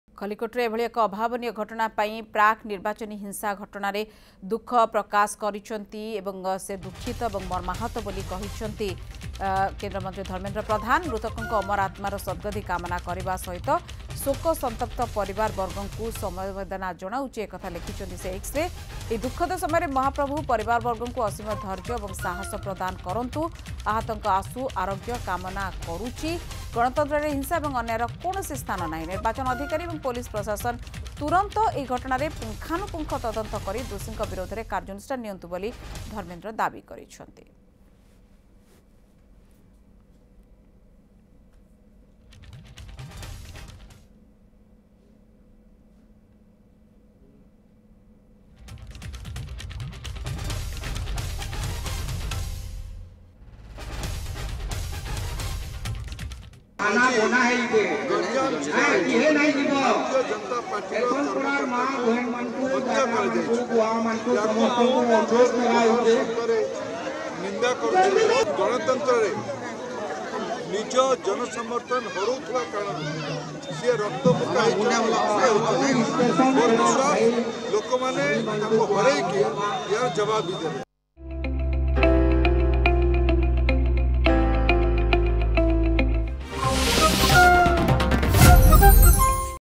I am saddened by the pre-poll violence:Dharmendra|ପ୍ରାକ୍ ନିର୍ବାଚନୀ ହିଂସା ଘଟଣାରେ ମୁଁ ଦୁଃଖିତ ଓ ମର୍ମାହତ
I am saddened by the pre-poll violence: Dharmendra Pradhan | ପ୍ରାକ୍ ନିର୍ବାଚନୀ ହିଂସା ଘଟଣାରେ ମୁଁ ଦୁଃଖିତ ଓ ମର୍ମାହତ: ଧର୍ମେନ୍ଦ୍ର ପ୍ରଧାନ